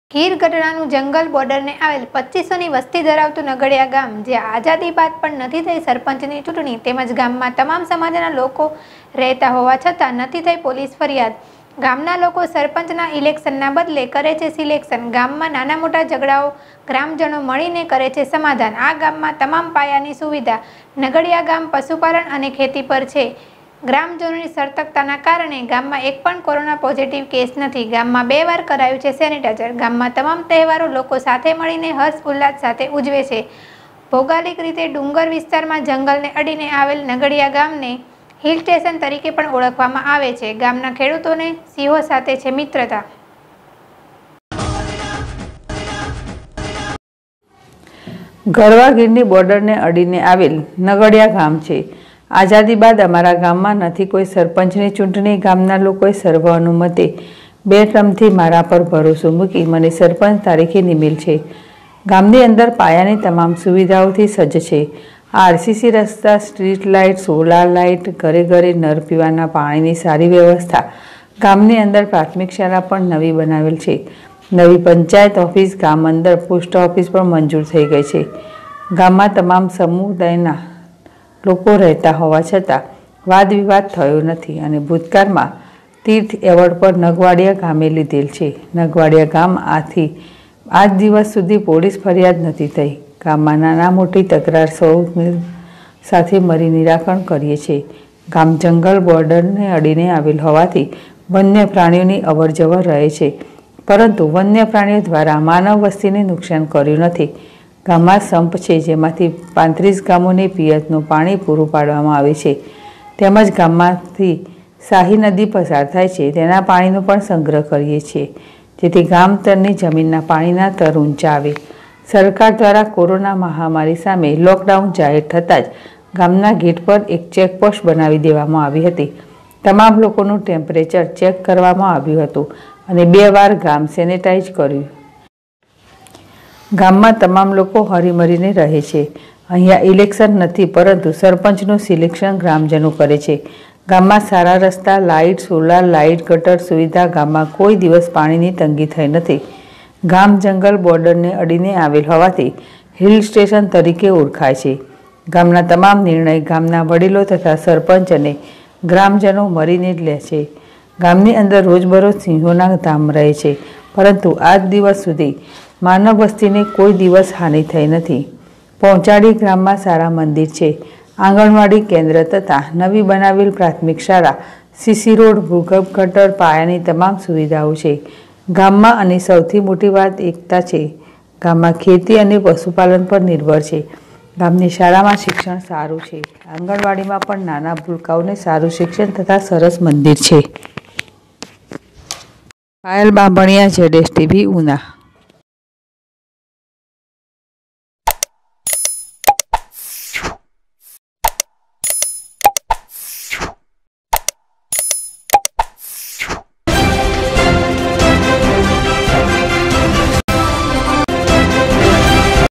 ગીરગઢડા નુ જંગલ બોર્ડર ને આવેલ 2500ની વસ્તી ધરાવતુ નગડીયા ગામ જ્યા આઝાદી બાદ નથી થઈ સરપંચ
ગીરગઢડા નુ જંગલ બોર્ડર ને આવેલ 2500ની વસ્તી ધરાવતુ નગડીયા ગામ જ્યા આઝાદી બાદ નથી થઈ સરપંચ ની ચુટણી તેમજ ગામ મા તમામ સમાજ ના લોકો રહેતા હોવા છતા નથી થઈ પોલીસ ફરીયાદ .ગામ ના લોકો સરપંચ ના ઈલેક્શન ના બદલે કરેછે સીલેકશન.ગામ મા નાના મોટા ઝગડા નો ગ્રામજનો મળી ને કરેછે સમાધાન.ગામ મા છે તમામ પાયા ની સુવિધા.